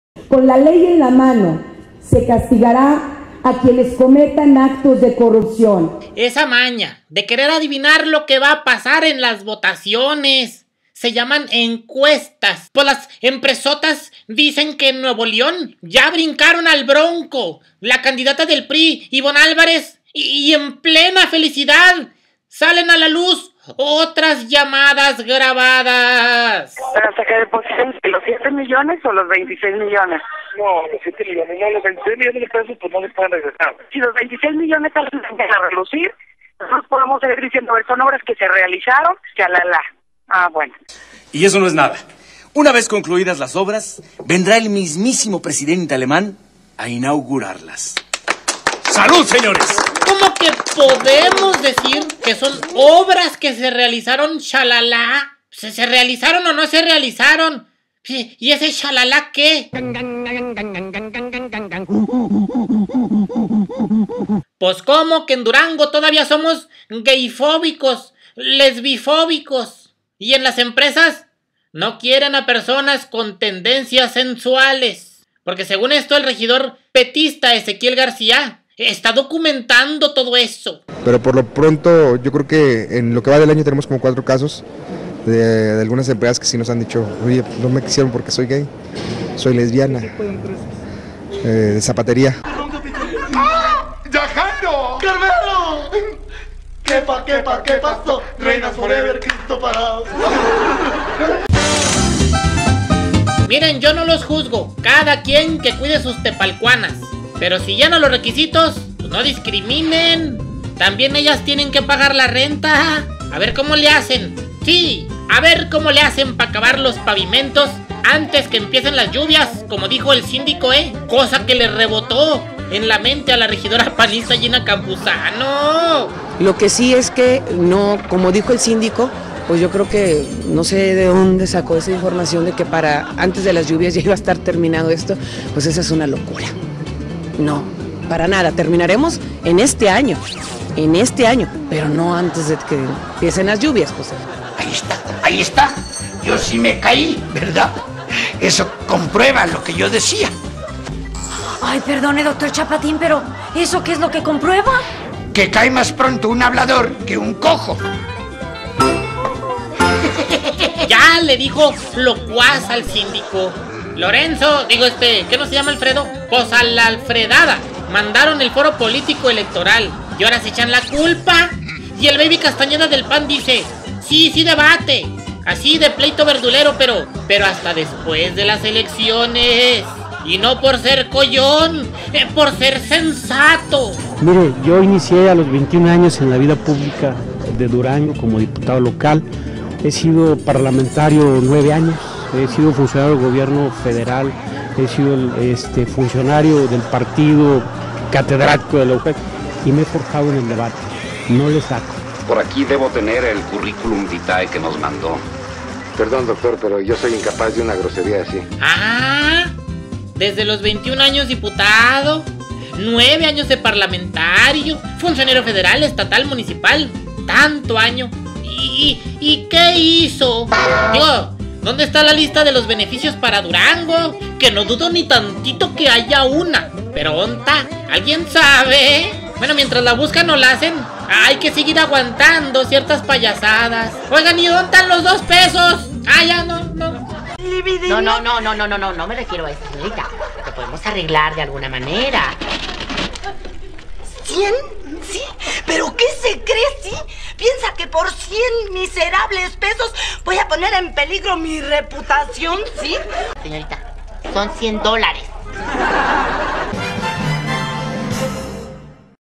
Llamadas grabadas
El Hueso en el Pescuezo.
Viernes 22 may 15
Voz Off: Esa maña de querer adivinar lo que va a pasar en las votaciones… se llaman encuestas… pos las empresotas dicen que en Nuevo León, ya brincaron al Bronco, la candidata del PRI Ivonne Álvarez y en plena felicidad salen a la luz, otras llamadas grabadas…
Voz Off: Cómo que podemos decir que son obras que se realizaron Shalalá? Se realizaron o no se realizaron… ese shalalá qué?...
Voz Off: Cómo que en Durango todavía somos gayfóbicos, lesbifóbicos, y que en las empresas no quieren a personas con tendencias sensuales, porque según esto el regidor petista Ezequiel García está documentando eso…
Ezequiel García, Regidor Capitalino. PT.
En lo que va del año tenemos como 4 casos de algunas empleadas que sí nos han dicho, oye pues no me quisieron porque soy gay, soy lesbiana… zapatería.
Voz Off: Yo no los juzgo, cada quien que cuide sus naves… pero si llena los requisitos, no discriminen, también ellas tienen que pagar la renta… a ver cómo le hacen, para acabar los pavimentos antes que empiecen las lluvias como dijo el síndico eh… cosa que rebotó en la mente a la regidora panista Gina Campuzano…
Gina Campuzano, Regidora Capitalina. PAN.
Como dijo el síndico, pues yo creo que… no sé de dónde sacó esa información de que para antes de las lluvias ya iba a estar terminado esto, pues esa es una locura… no, para nada…
Voz Off: Ya le dijo locuas al síndico, Lorenzo… qué no se llama Alfredo? Pos a la fregada mandaron el foro político electoral y ahora se echan la culpa… y el Baby Castañeda del PAN, dice que sí debate así de pleito verdulero, pero hasta después de las elecciones y no por ser coyón, sino por ser sensato…
Víctor Hugo Castañeda,
Mire, yo inicié a los 21 años en la vida pública de Durango como diputado local, he sido parlamentario 9 años, he sido funcionario de gobierno federal, ha sido funcionario del partido, catedrático de la UJED y me he forjado en el debate, no le saco.
Voz Off: Ah, desde los 21 años diputado, 9 años parlamentario, funcionario federal, estatal, municipal tanto año… y qué hizo? Digo dónde está la lista de beneficios para Durango? Que no dudo ni tantito que haya una, pero on ta?... alguien sabe?, mientras la buscan o la hacen hay que seguir aguantado ciertas payasadas… on tan los 2 pesos?